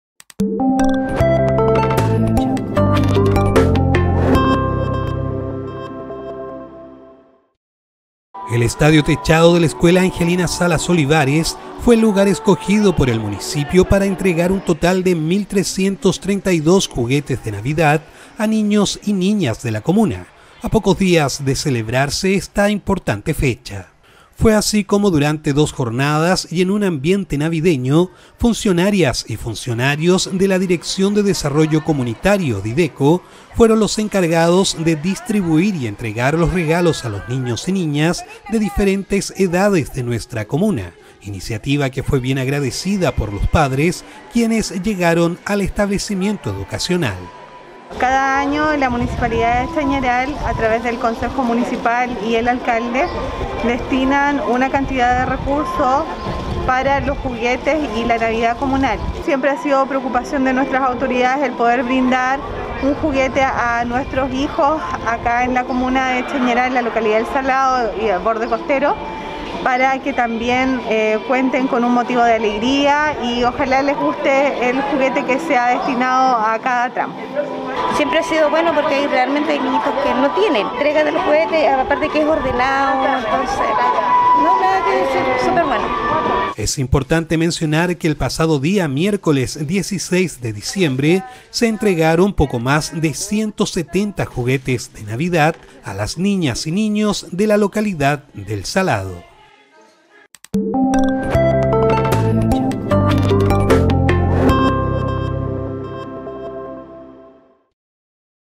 MUNICIPIO DE CHAÑARAL ENTREGÓ 1.332 JUGUETES DE NAVIDAD
El gimnasio techado de la escuela Angelina Salas Olivares, fue el lugar escogido por el municipio para entregar un total de 1.332 juguetes de Navidad a niños y niñas de la comuna, a pocos días de celebrarse esta importante fecha.

Fue así como durante las jornadas de jueves y viernes y en un ambiente navideño, funcionarias y funcionarios de la Dirección de Desarrollo Comunitario (DEDICO), fueron los encargados de distribuir y entregar los regalos a los niños y niñas de diferentes edades de nuestra comuna, iniciativa que fue bien agradecida por los padres quienes llegaron al establecimiento educacional.

Nuestro Alcalde Raúl Salas Aguilera, comentó que "nuestro deseo es que todas y todos los niños tengan una linda Navidad, en un 2020 que ha sido significativamente diferente por la pandemia que estamos atravesando, pero sabemos que iniciativas como estás son muy agradecidas por nuestra comunidad y eso como administración municipal nos pone muy contentos".

Es importante mencionar, que el pasado día miércoles 16 de diciembre, se entregaron un poco más de 170 juguetes de Navidad a las y los niños de El Salado.